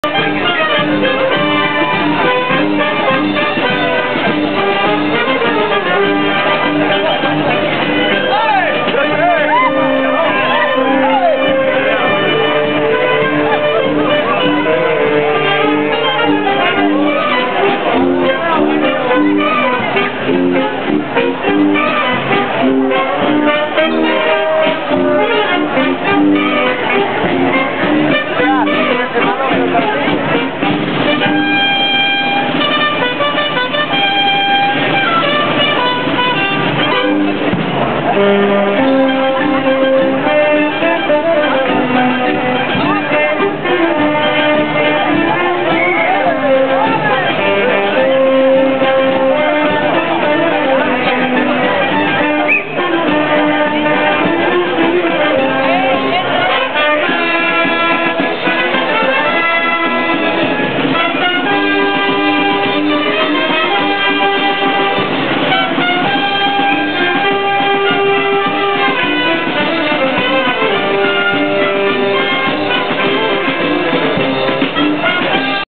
Feria de Malaga 2008 (I)
Band playing and people dancing at Feria de Malaga 2008